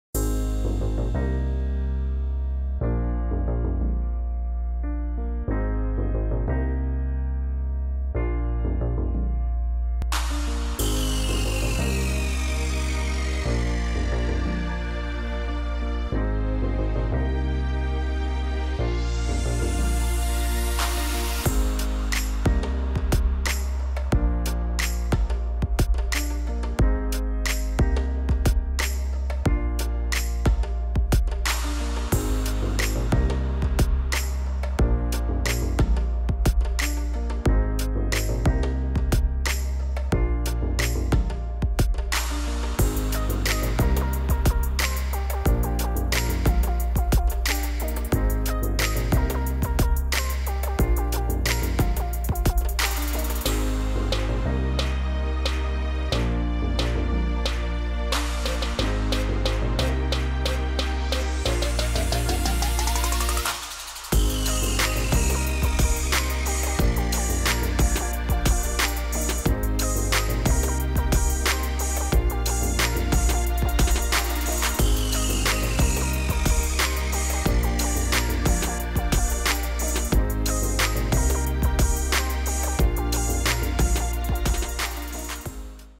Drawing Mikayla Nogueira (Famous TikToker Timelapse)
Lucy is a huge fan of Mikayla Nogueira, so why not just draw her?